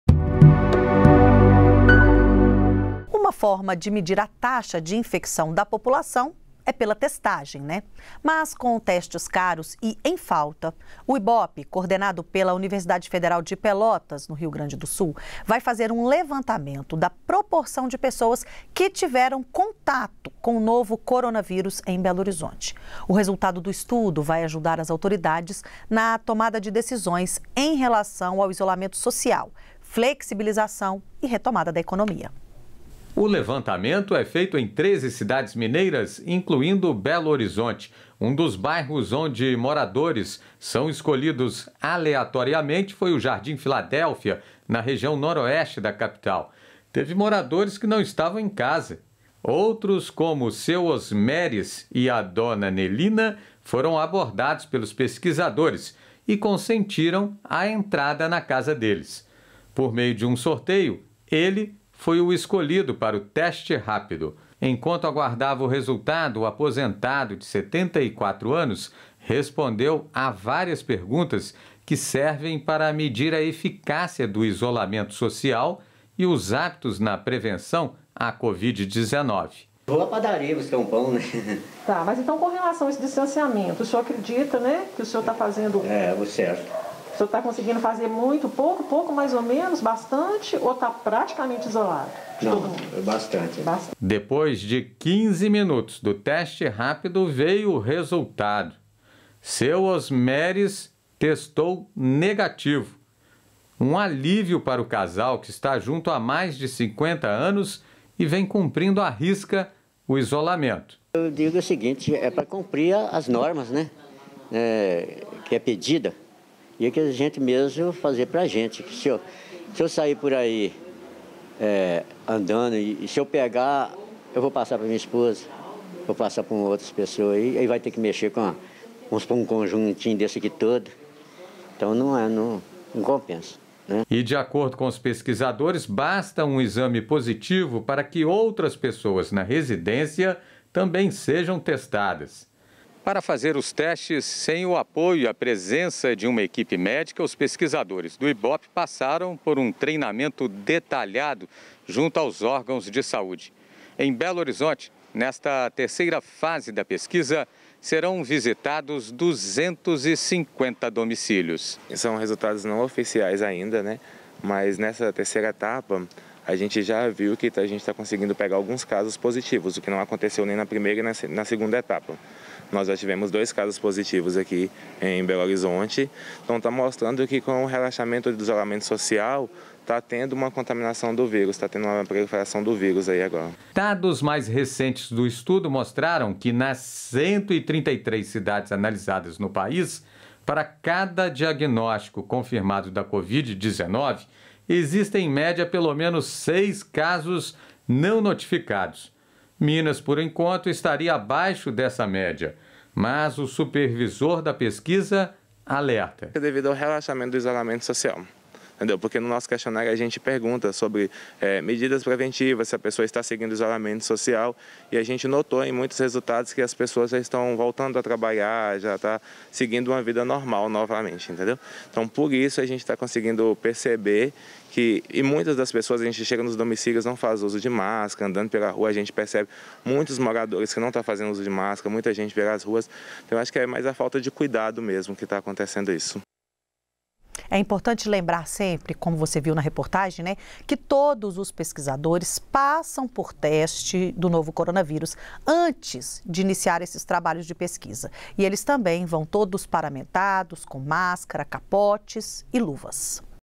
IBOPE FAZ PESQUISA SOBRE CORONAVÍRUS EM BH - Jornal Minas
Com testes caros e em falta, o Ibope, coordenado pela Universidade Federal de Pelotas (UFPel), no Rio Grande do Sul, vai fazer um levantamento da proporção de pessoas que tiveram contato com o coronavírus na capital. O resultado vai ajudar as autoridades na tomada de decisões em relação ao isolamento social, flexibilização e na retomada da economia.